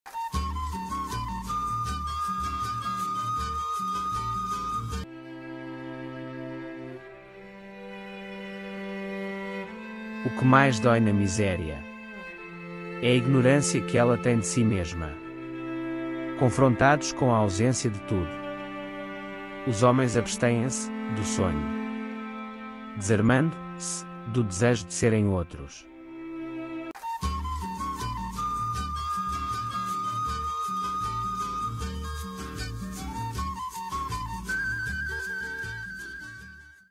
Poema O QUE MAIS DÓI NA MISÉRIA Mia Couto
O poema de Mia Couto trata sobre a dor e a desolação causadas pela miséria. Ele mostra como a miséria pode levar os seres humanos a um estado de desespero e desolação, onde as suas próprias esperanças e sonhos são destruídos. O poema destaca a solidão e o desespero que acompanham a miséria, e como ela pode deixar as pessoas sem nada, sem ninguém para consolá-las. Ao mesmo tempo, o poema também destaca a força e a determinação que as pessoas possuem para lutar contra a miséria e encontrar um caminho para um futuro melhor.